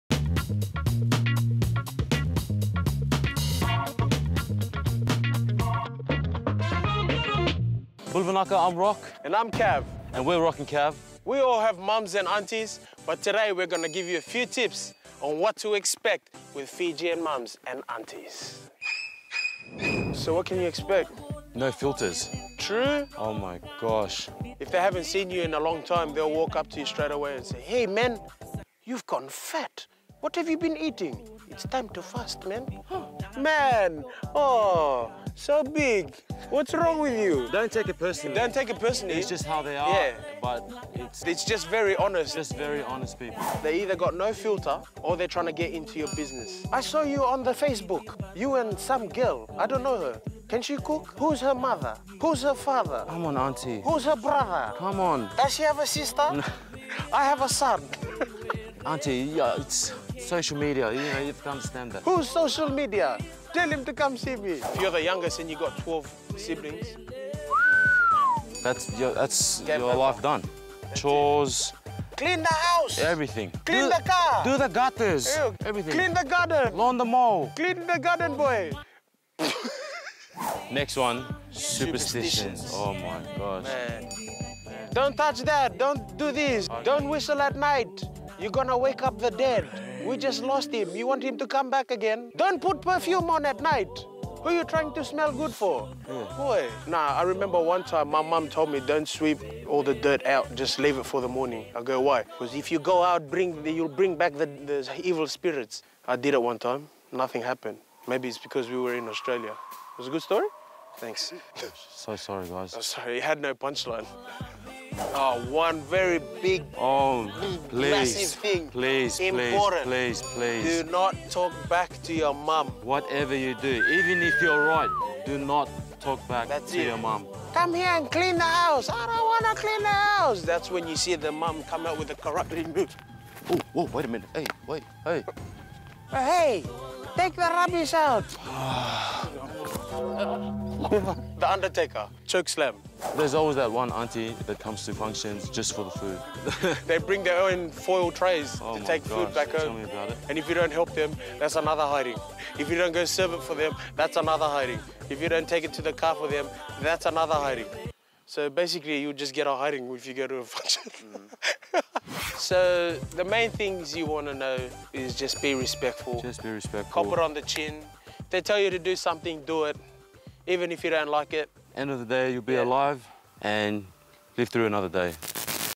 ROQ AND CAV - FIJIAN FRESH TIPS TO SURVIVING MAMA SHADE
Roq & Cav share how to survive the Mums and Aunties', tough love.
Our fave Fijians Roq & Cav are back with their fresh tips on what to expect from Fijian Mums & Aunties!